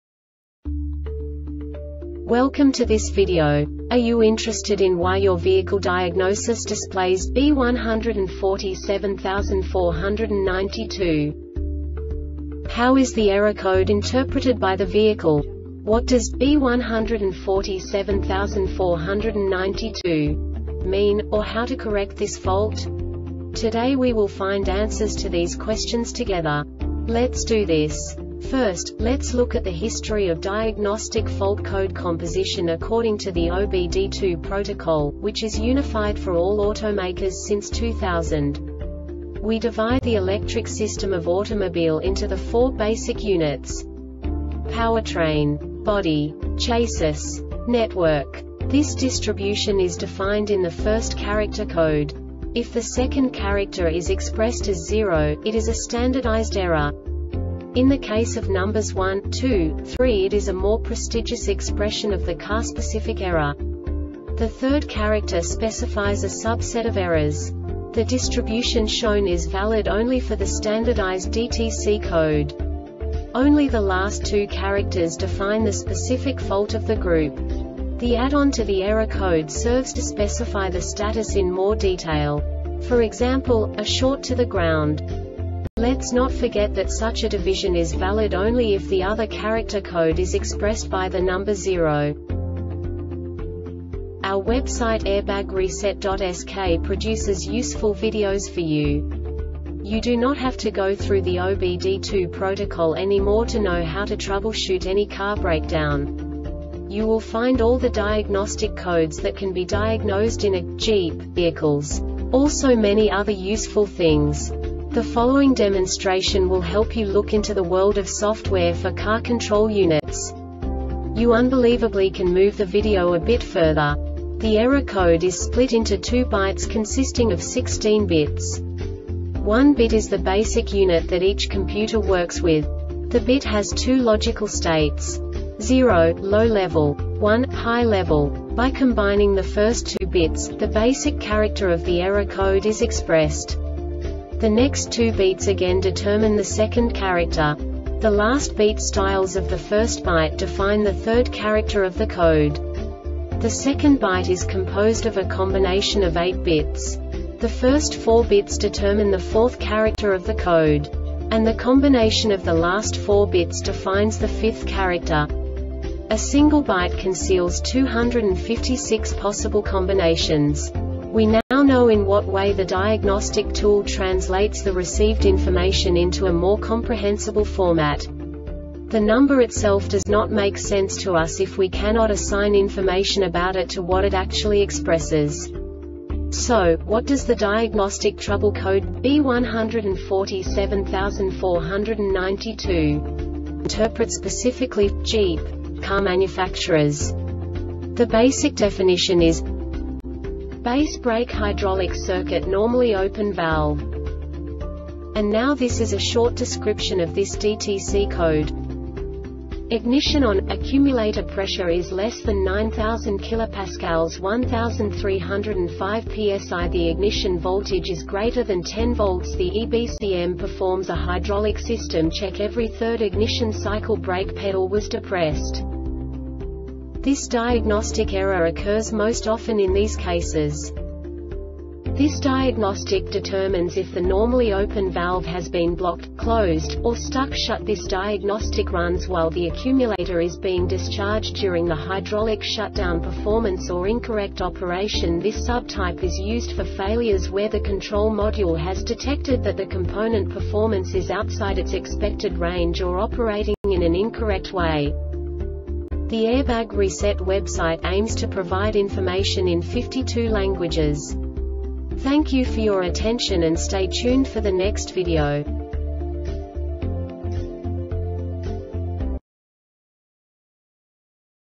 DTC Jeep B1474-92 Short Explanation
Contents:
0:21 Basic DTC analysis according to OBD2 protocol standard.
1:48 Insight into programming
2:58 A diagnostically understandable description of the Diagnostic Trouble Code
3:05 Basic error definition

Make:
Jeep
Code:
B1474 92
Definition:
Base Brake Hydraulic Circuit Normally Open Valve
Description:
Ignition ON,accumulator pressure is less than 9,000 kPa (1,305 psi).The ignition voltage is greater than 10 volts.The EBCM performs a hydraulic system check every third ignition cycle.Brake pedal was depressed.
Cause:
This diagnostic determines if the normally open valve has been blocked,closed,or stuck shut.This diagnostic runs while the accumulator is being discharged during the hydraulic shutdown.